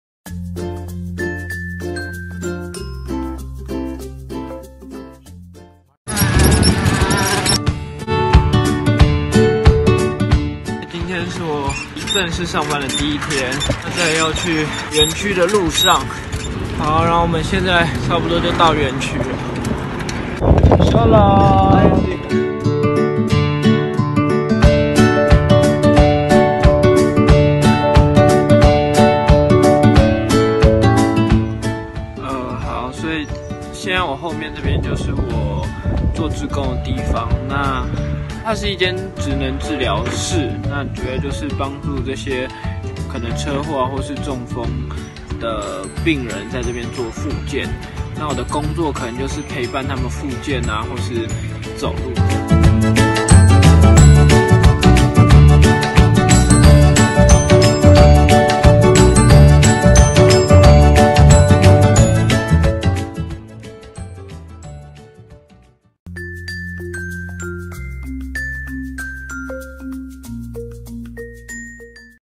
我在以色列的ALEH做服務
ALEH是以色列最主要的多重殘疾兒童護理機構。 ALEH與來自世界各地的殘疾兒童的父母、工作人員、志工和捐贈者合作，改善了以色列四個機構中700多名兒童的生活。